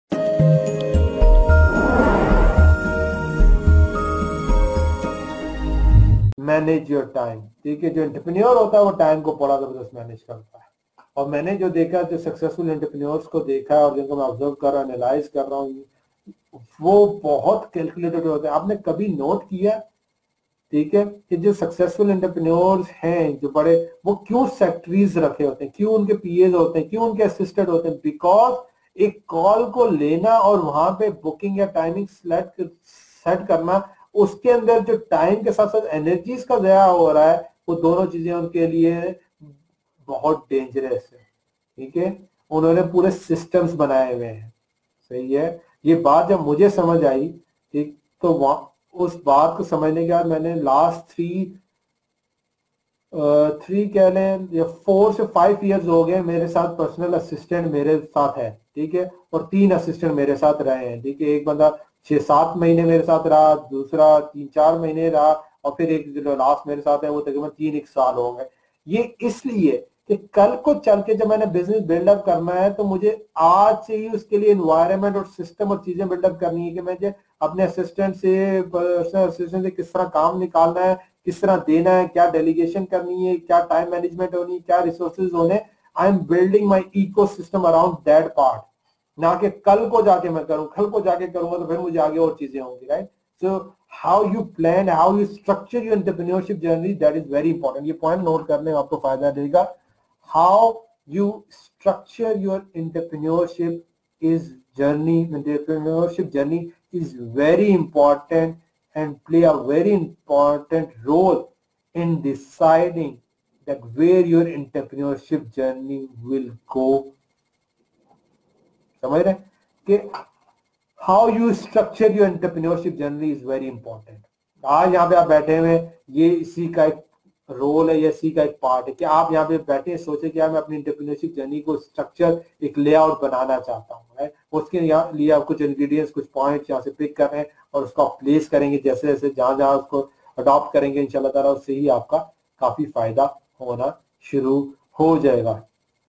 How To Manage Your Time?
Managing Time & How You Structure Your Entrepreneurship Journey is Very Important. This will decide where you are Heading in terms of Growth.